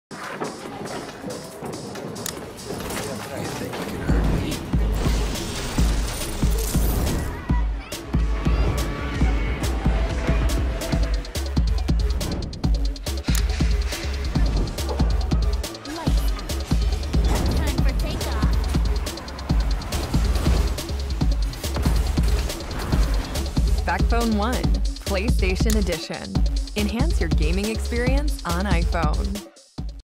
Backbone One - PlayStation Edition Trailer
Check out the trailer for Backbone's newest controller, the Backbone One - PlayStation Edition. Inspired by Sony's DualSense controller, this is an officially licensed controller, created for players to hop into their PlayStation games on the go, using their iPhone and PS Remote Play.